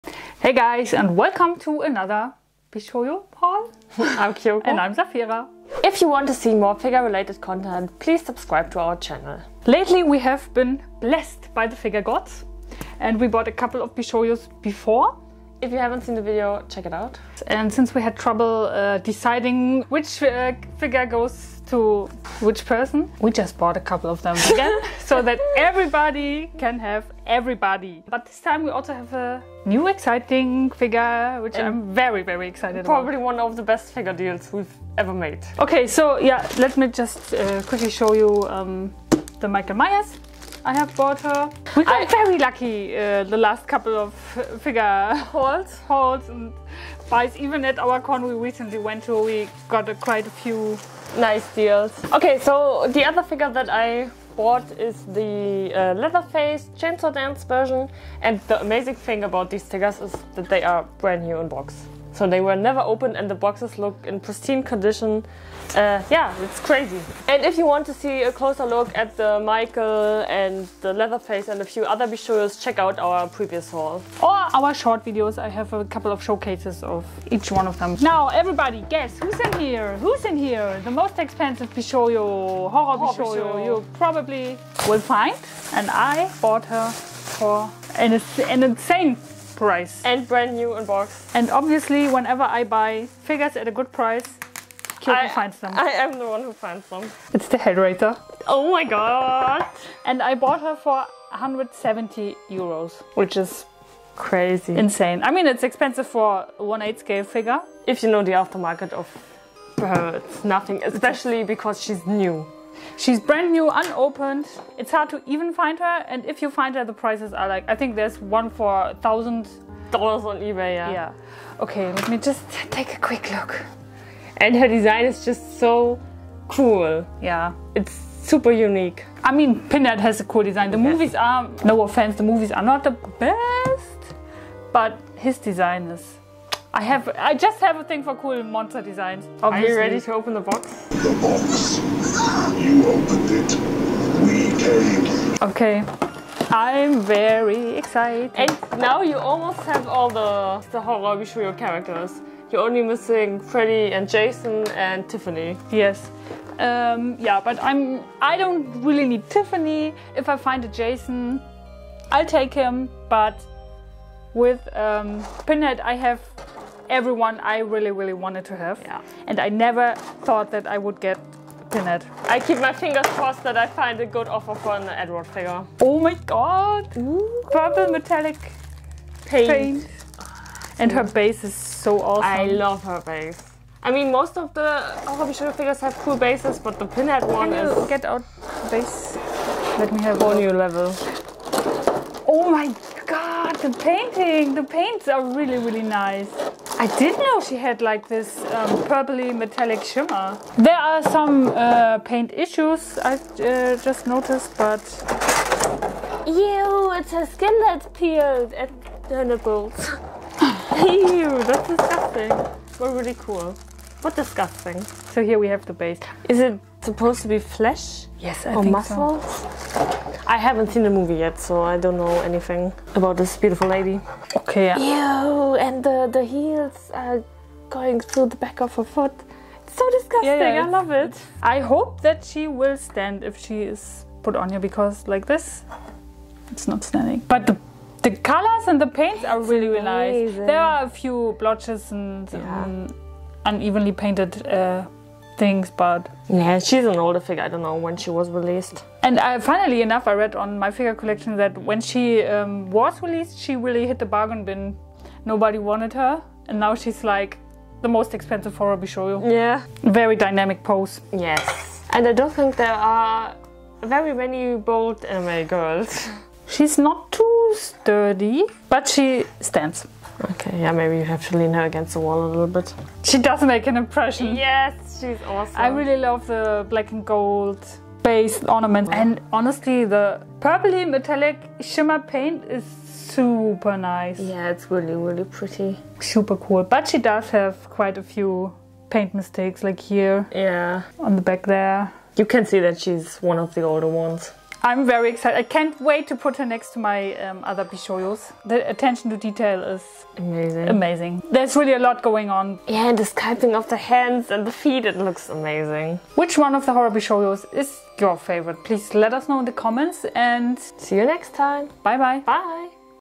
More HORROR Bishoujos?!?// Anime Figure Unboxing & Haul
In this video we are taking a look at even more horror bishoujo figures from Kotobukiya. We are also unboxing the Pinhead Bishoujo from Hellraiser.